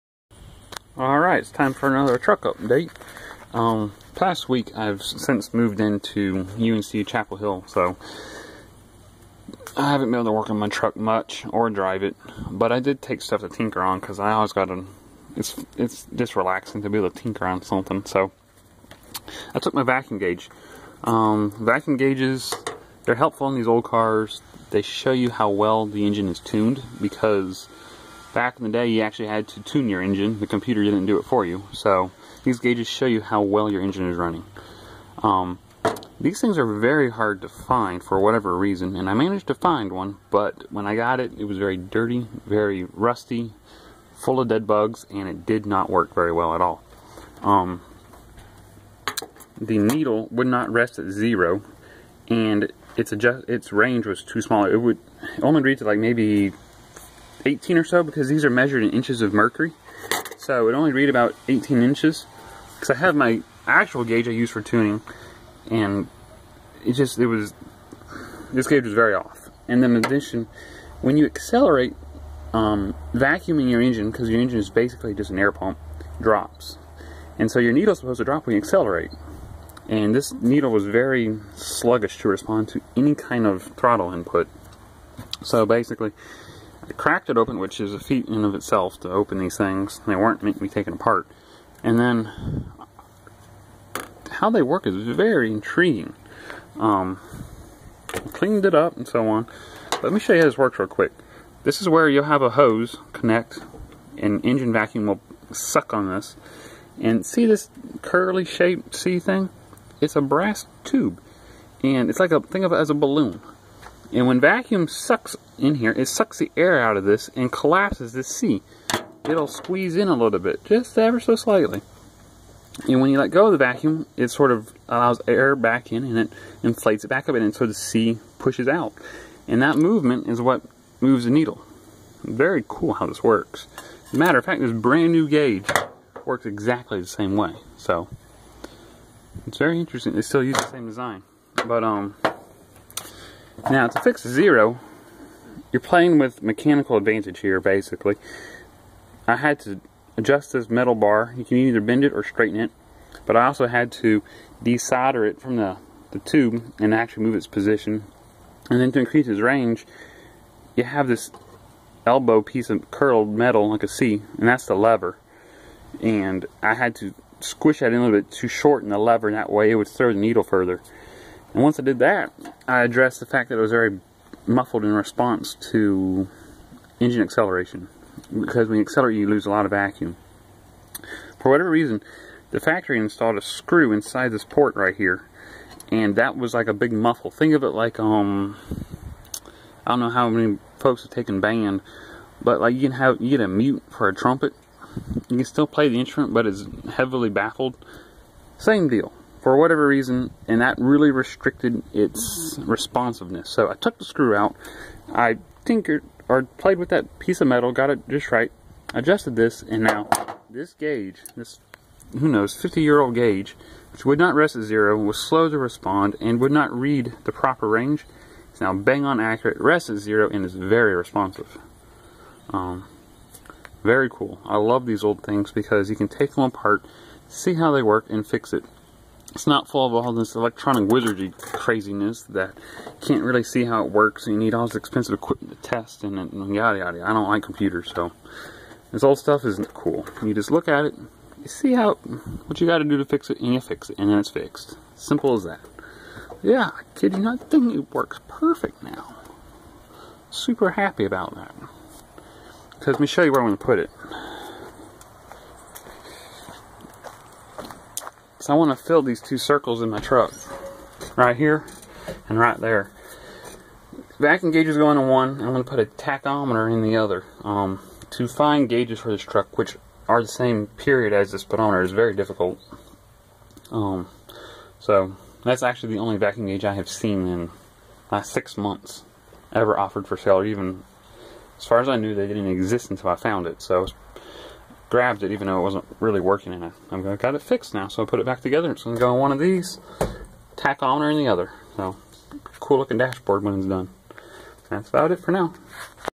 Vacuum Gauge Repair -- 1966 Ford F100 #19
I've been working on a vacuum gauge lately.